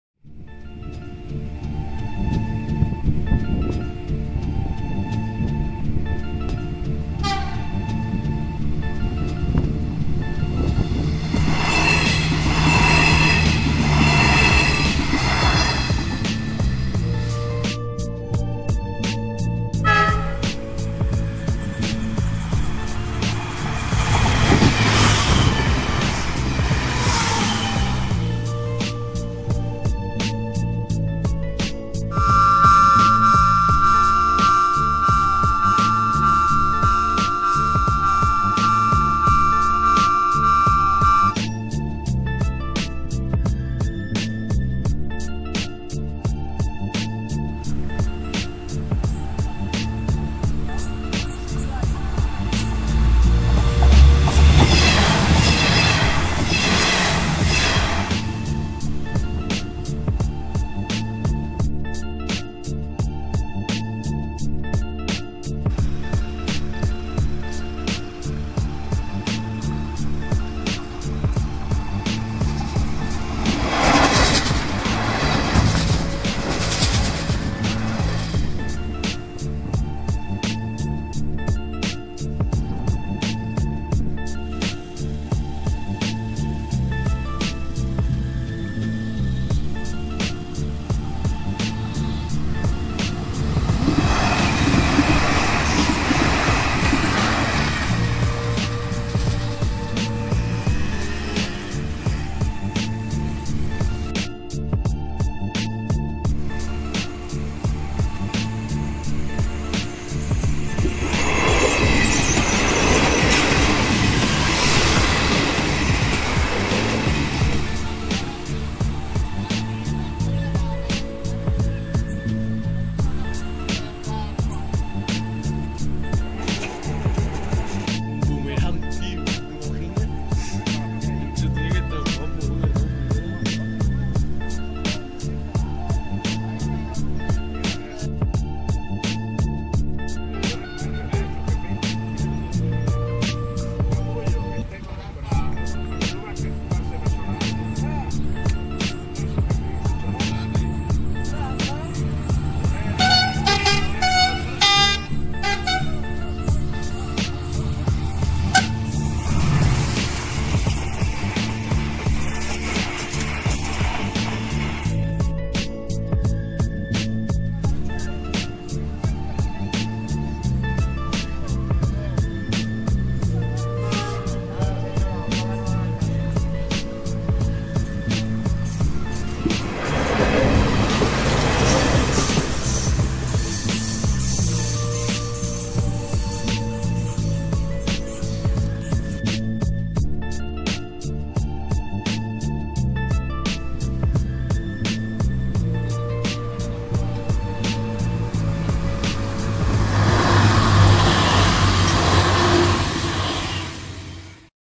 Últimos trenes - PN Senda De Los Garres
Martes 22 de Mayo de 2018, se cierra permanentemente el paso a nivel de Senda De Los Garres por las obras del AVE, entre las 20:15 y las 21:30 @bromley_01 (@jorgethespotter) y yo fuimos a grabar algunos de los últimos servicios en atravesar el PN aún en activo, pocas horas después, se cerraba definitivamente a las 00:00

FLICKR:trenale